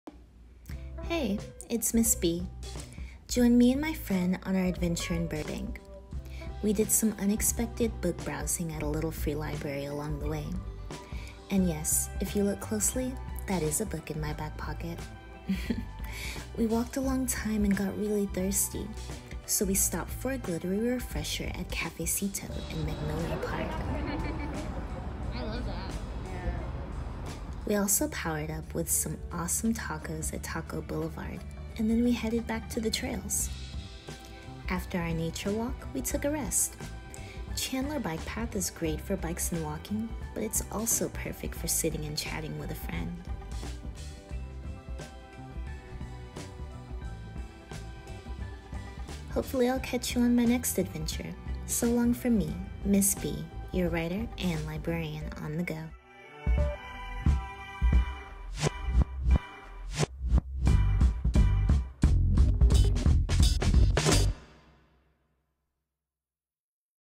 Adventures In Burbank: Magnolia Park, Cafecito, Chandler Bike Path
Chill things to do on an afternoon in Burbank.